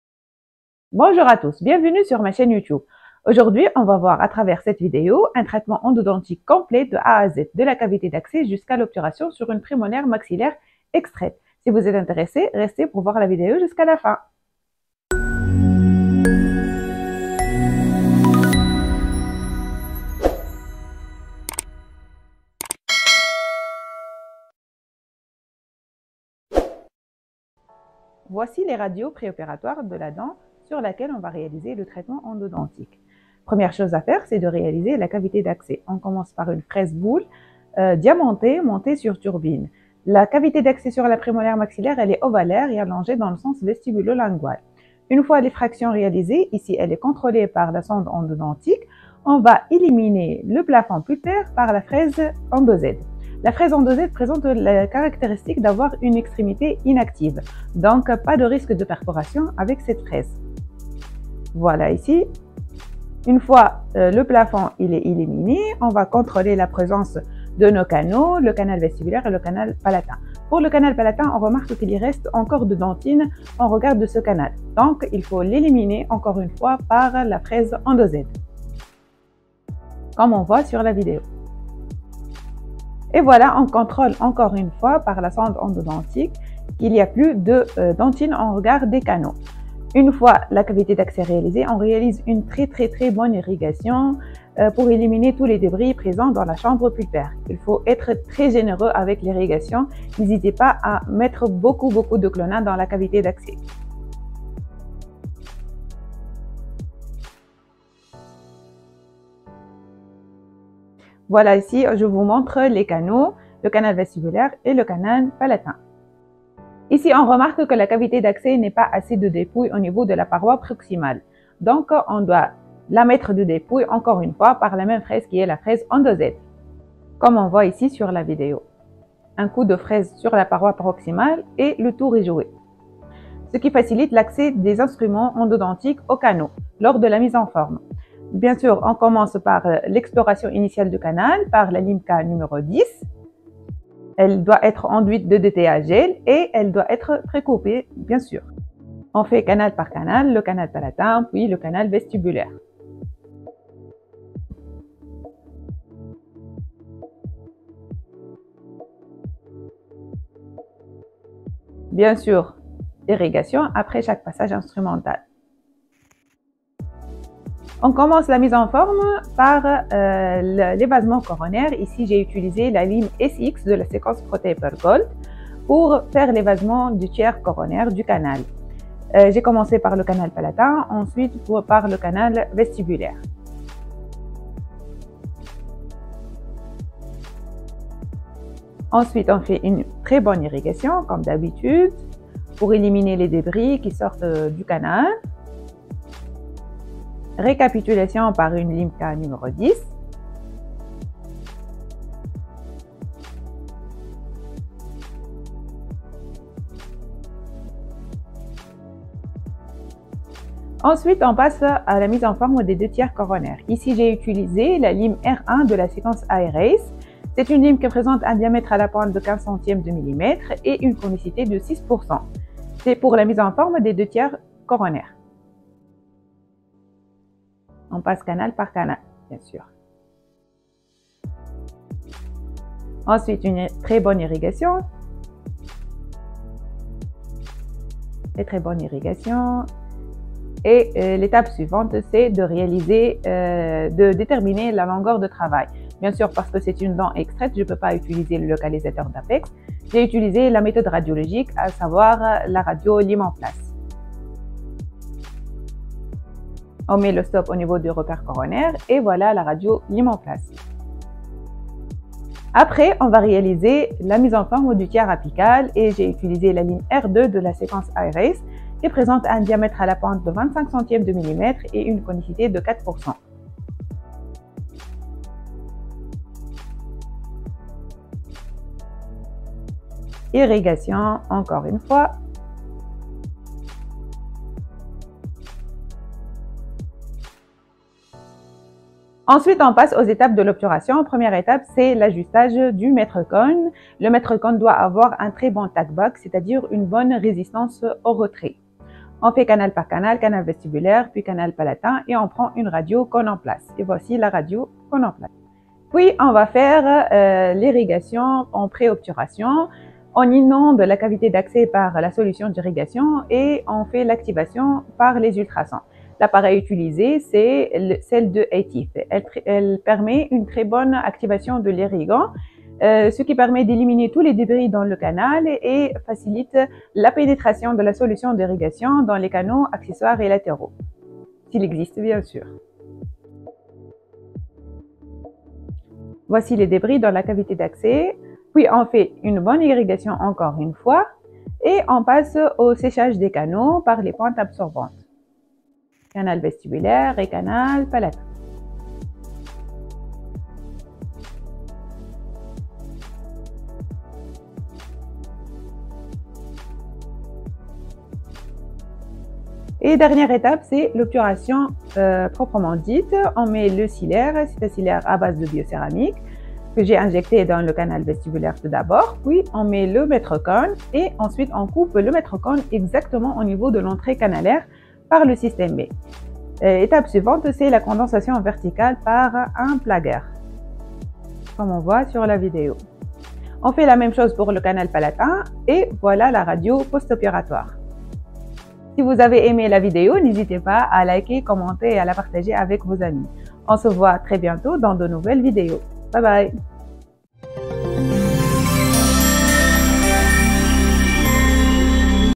Root canal treatment from A to Z: Step by Step
Dans cette video, on va voir les étapes d'un traitement endodontique complet de A à Z: c'est à dire de la cavité d'accès jusqu'à l'obturation canalaire définitive.

Materiels utilisés:

- Cavité d'accès : fraise boule diamentée, fraise endoZ, sonde endodontique

-Mise en forme: SX (protaper gold), R1 et R2 ( irace de FKG)

-Obturation: sealer a base de bioceramiques